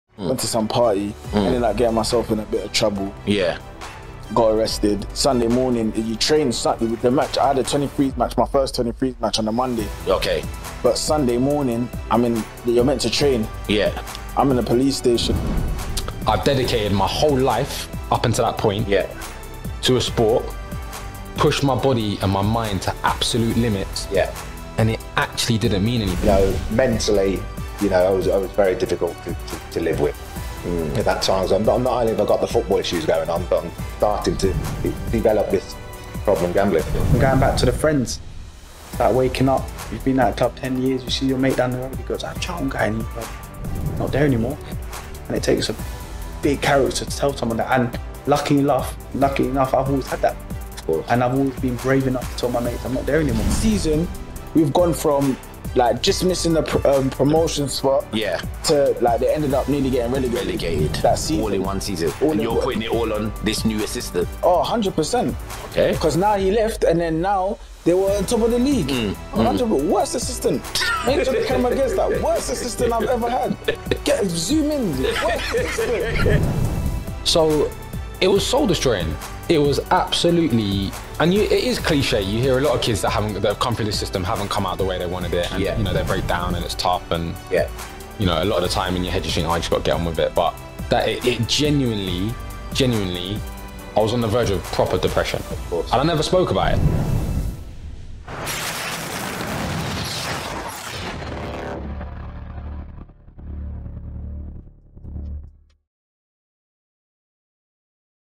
My Truth - Promotional Video - Series 1 starts Friday 30th July
The Brand New Podcast where we take a deep dive insight into the Journeys & Experiences of Young men & women on the way to becoming professional sports people. The stresses on family life and the strain on one's mental health. These series of shows are educational, emotional and entertaining, we hope you enjoy watching as much as we enjoyed creating MY TRUTH